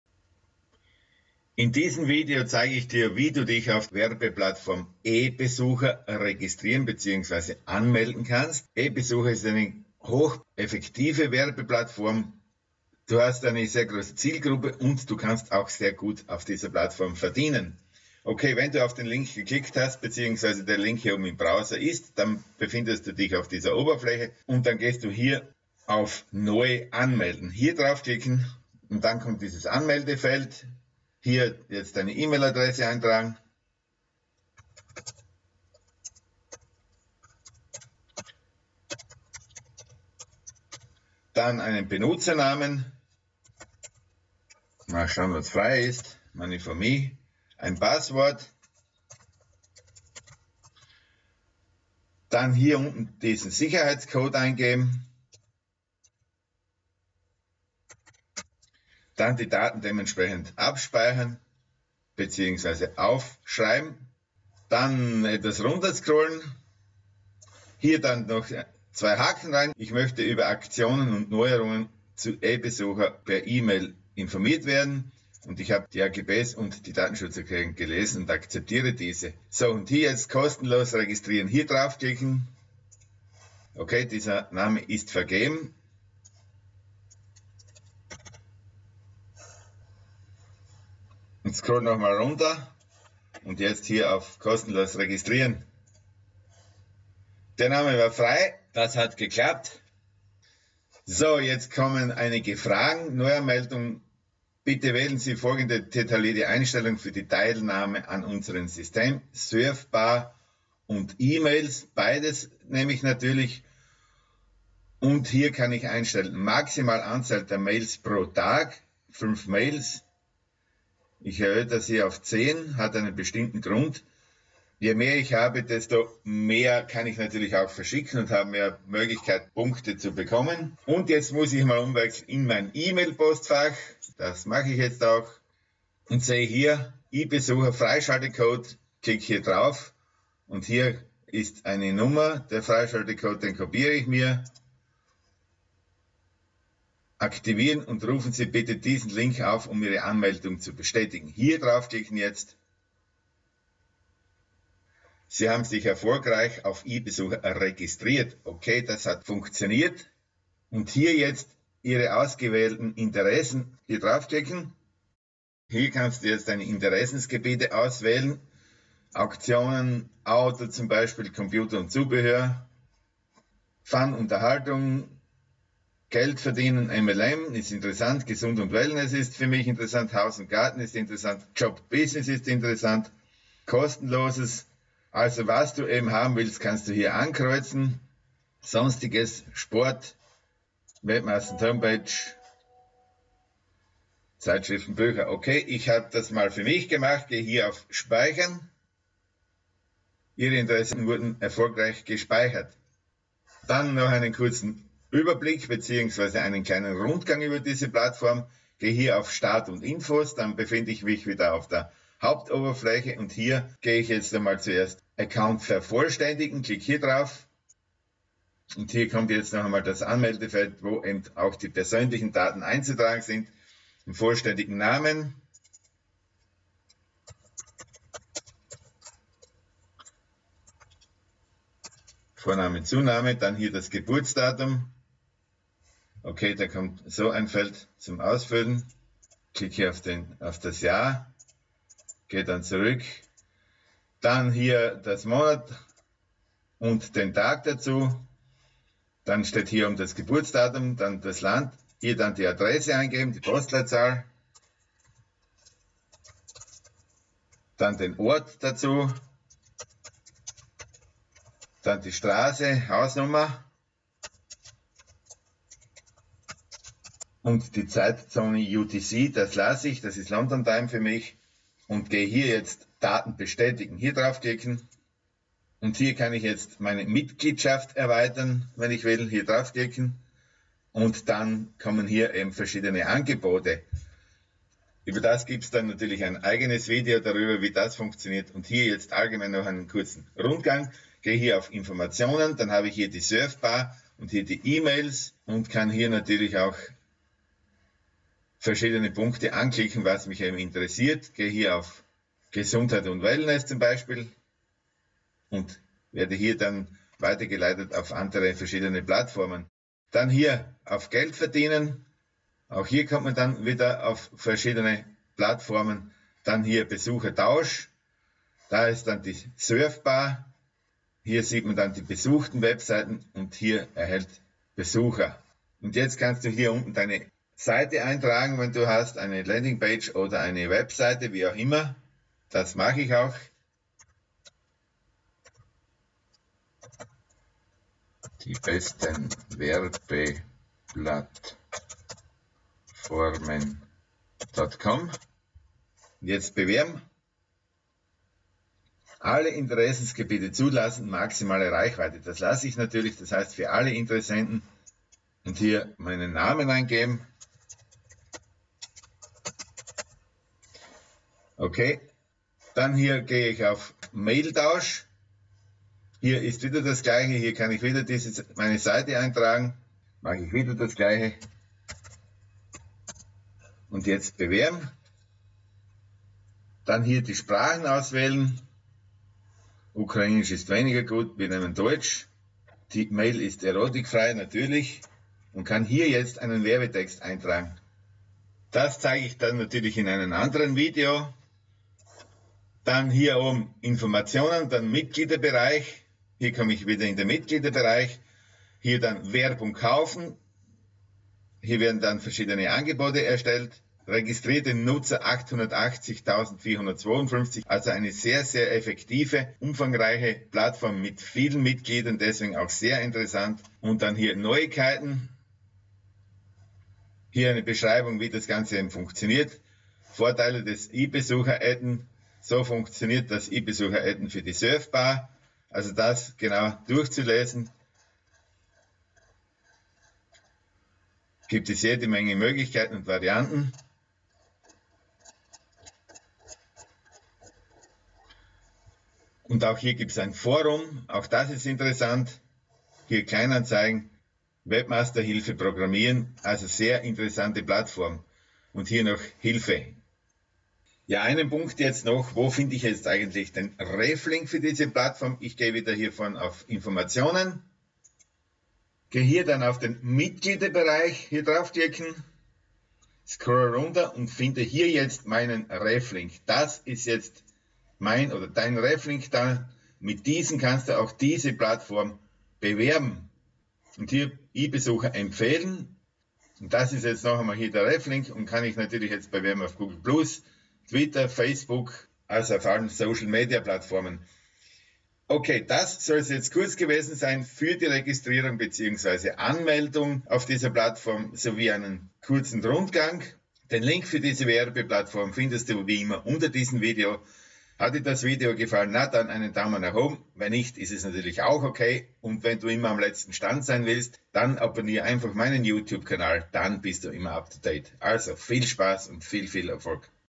Registrierung auf der Werbeplattform "Ebesucher"! Online Geld verdienen!
Hier zeig ich dir wie du dich auf dieser Werbeplattform registrieren  bzw. anmelden kannst, sowie einen kurzen Überblick über diese Plattform!

Hier den Zugang zu noch weiteren interessanten Werbeplattformen!
Hier den Zugang zu noch weitere interessante Programme und Produkte!